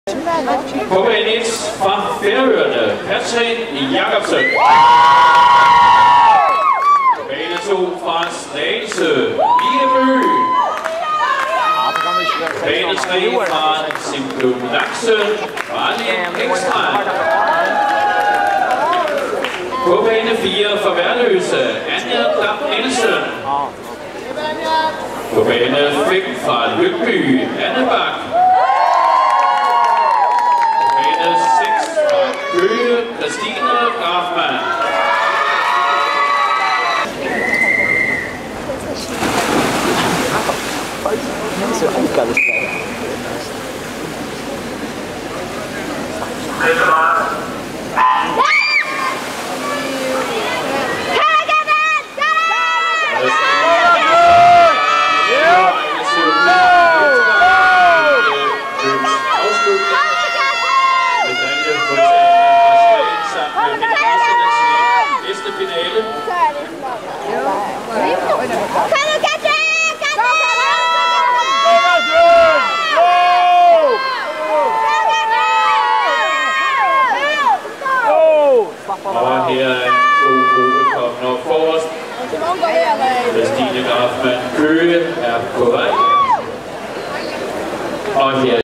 Køge Open 2008 - 50 rygg finalu hjá kvinnum - riðil 4 (partur 56 av 73)
26. oktober kl 16.13.25

Riðil 4

Geil 1 - Katrin Jacobsen (1992), FLOT
Geil 2 - Mie Bøgh (1992), Slagelse Svømmeklub
Geil 3 - Malin Ekstrand (1991), Simklubben Laxen
Geil 4 - Anja Dam-Nielsen (1992), Værløse Gymn & Svømmeklub
Geil 5 - Anne Bach (1991), Lyngby Svømmeklub
Geil 6 - Kristine Graffmann (1991), Køge Svømmeklub